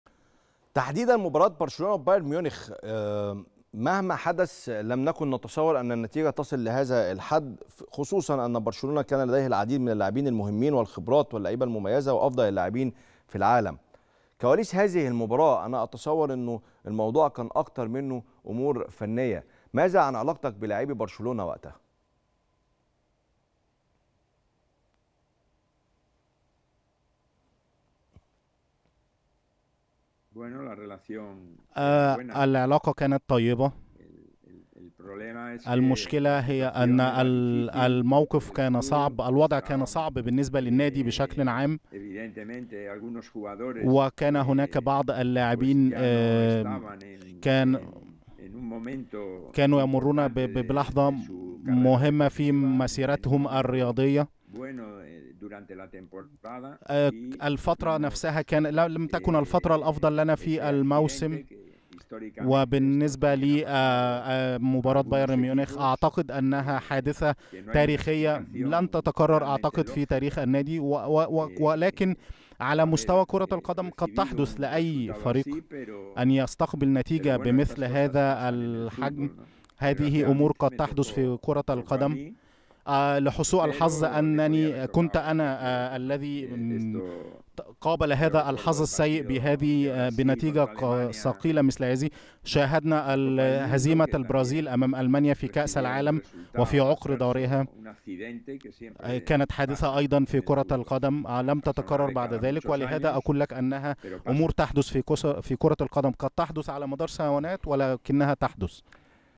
كيكي ستين مدرب برشلونة السابق يتحدث عن كواليس الهزيمة التاريخية لبرشلونة من البايرن بنتيجة ٨-٢🔥💥
خاااااااص وحصري .. كيكي ستين مدرب برشلونة السابق يتحدث عن كواليس الهزيمة التاريخية لبرشلونة من بايرن ميونيخ بنتيجة ٨-٢🔥💥